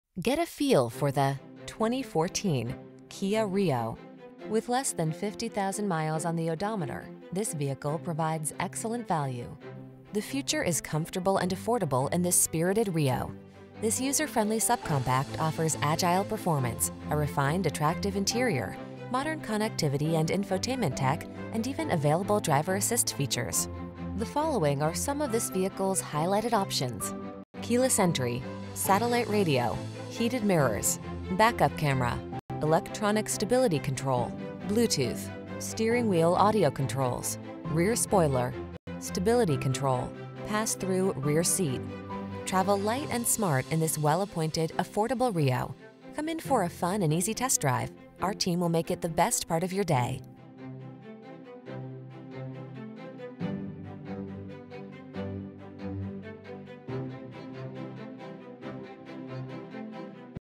2014 Kia Rio Gilbert, Chandler, Mesa, Queen Creek, San Tan, AZ K7741
Clear White Used 2014 Kia Rio available in Gilbert, Arizona at Horne Kia. Servicing the Chandler, Mesa, Queen Creek, San Tan, AZ area.

Horne Kia
1465 E Motorplex Loop